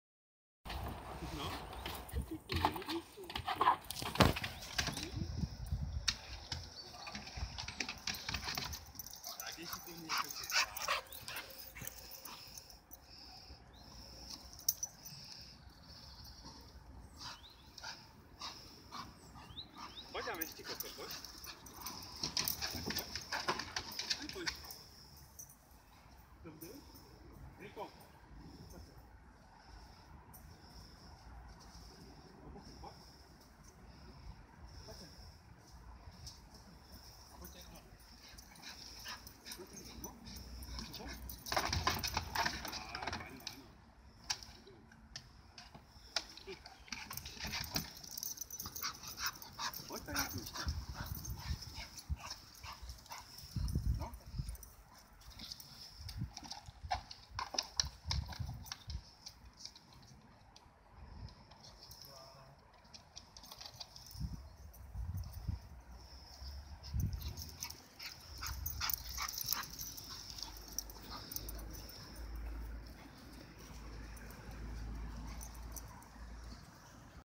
Valentýnské venčení a agility :) / Valentine´s day dog walking and agility :)
Vzali jsme Rica opět na agility překážky, aby se pocvičil :)

Místo: Brno-Medlánky